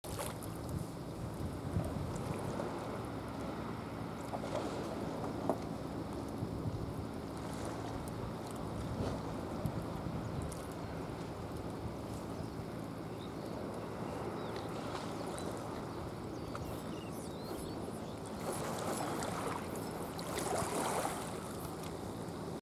９月５日 メッキの群れ
メッキはイワシ等に比べて動きが早いのが特徴。